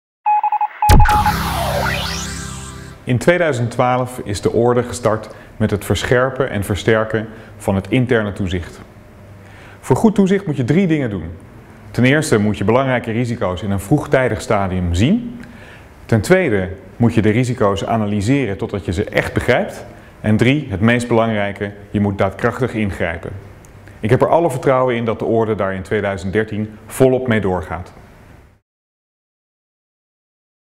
Adviseur Hanzo van Beusekom over toezicht binnen de advocatuur
In deze video vertelt extern adviseur toezicht Hanzo van Beusekom over het huidige en toekomstige toezicht op de advocatuur. Deze video behoort bij het Jaarverslag 2012 van de Nederlandse Orde van Advocaten.